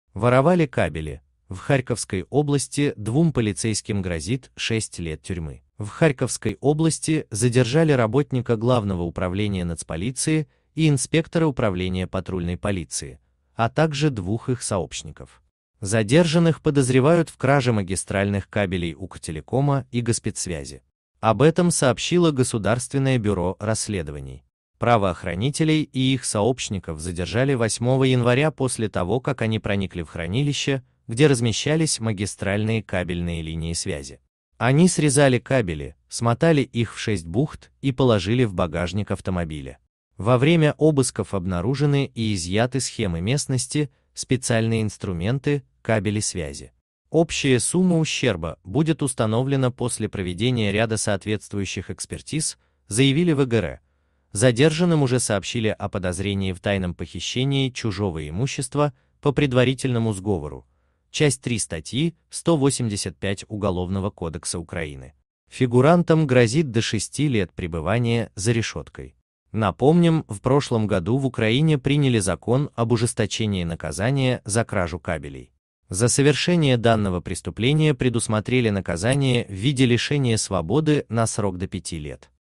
Воровали кабели: в Харьковской области двум полицейским грозит 6 лет тюрьмы.
Общество Харьков полиция кража ГБР Подробней на В Харьковской области задержали работника главного управления Нацполиции и инспектора управления патрульной полиции, а также двух их сообщников. Задержанных подозревают в краже магистральных кабелей "Укртелекома" и Госспецсвязи. Об этом сообщило Государственное бюро расследований. Правоохранителей и их сообщников задержали 8 января после того, как они проникли в хранилище, где размещались магистральные кабельные линии связи. Источник